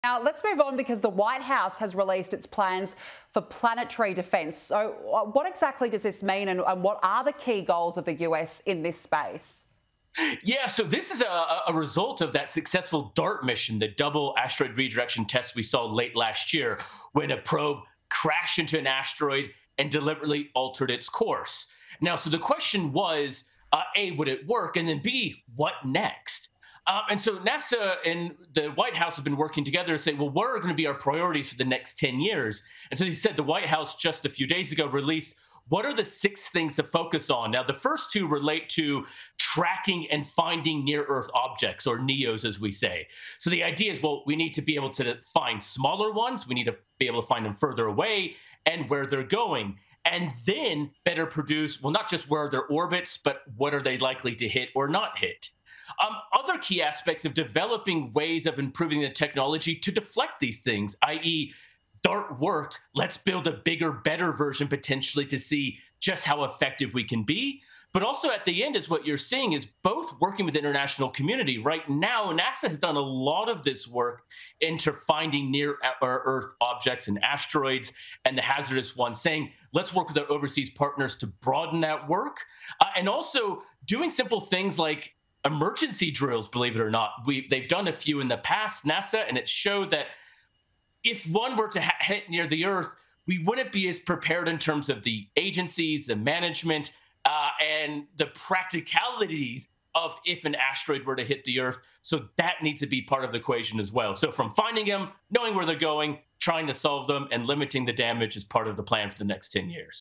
White House and NASA work together for new Planetary Defence Strategy
The White House has begun working with NASA for a new planetary defence strategy involving “tracking and finding near earth objects” and “developing ways of improving the technology,” says ANU Astrophysicist and Cosmologist Brad Tucker.

“This is a result of that successful DART mission … when a probe crashed into an asteroid and deliberately altered its course,” he told Sky News Australia.

“If one were to hit near the earth, we wouldn’t be as prepared in terms of the agencies, the management and the practicalities of if an asteroid were to hit the earth, so that needs to be part of the equation as well.

“Finding them, knowing where they’re going, trying to solve them and limiting the damage is part of the plan for the next ten years.”